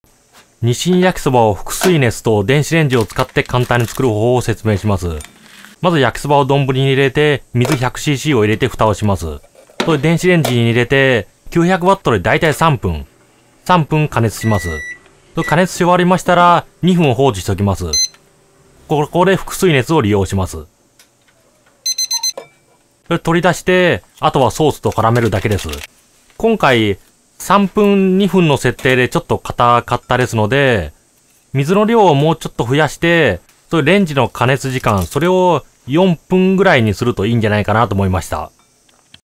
[解放軍]日清焼そばを電子レンジ＋蒸気の力で調理
意外に作るのが面倒な日清焼そばを電子レンジと蒸気の力で調理しました。　大きなどんぶりと蓋、少々の水がキーポイントです。　あと、加熱調理が終わっても蒸気が復水するまで待つのがキーポイントです。蒸気が水に戻るときに復水熱と呼ばれる熱が発生し、それを利用すると省エネで熱くありません。